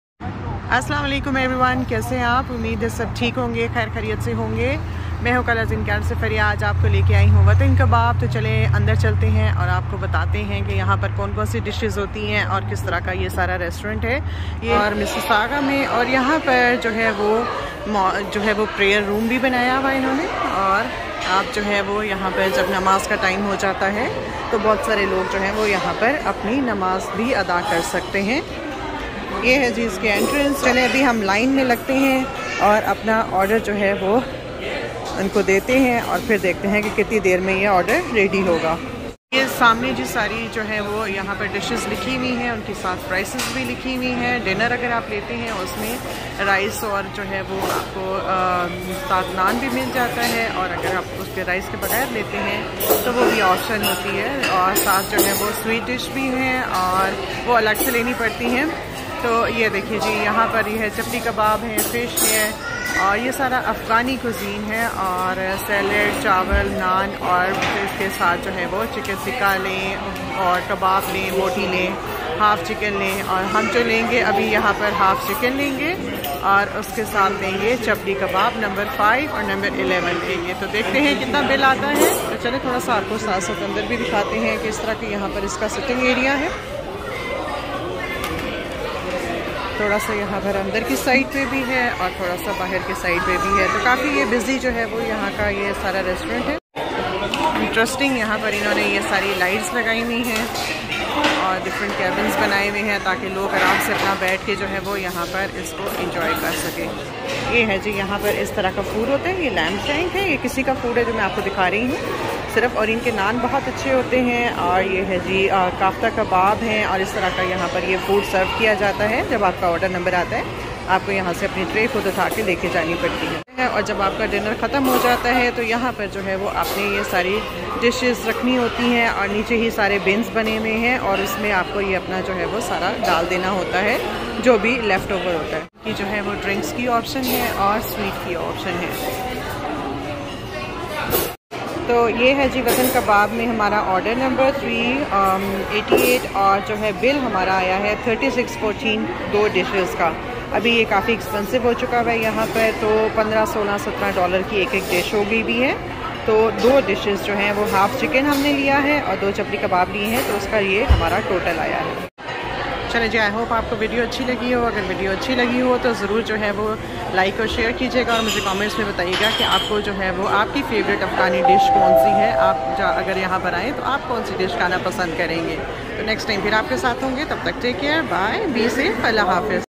“Experience the Rich Flavors of Afghanistan at Watan Kabob”
watankabob we explore the rich flavors of Afghanistan at Watan Kabob. From mouth-watering kabobs to traditional rice dishes, we’ll show you why this restaurant is a must-try in Mississauga
Looking for authentic Afghan cuisine? Look no further than Watan Kabob! In this video, we’ll take you on a culinary journey through the heart of Afghanistan.”
Get ready to experience the best Afghan grill in Mississauga In this video, we’ll introduce you to Watan Kabob and all the delicious dishes it has to offer.”
Watan Kabob is more than just a restaurant - it’s an experience. Join us as we discover the sights, sounds, and flavors of this amazing eatery.”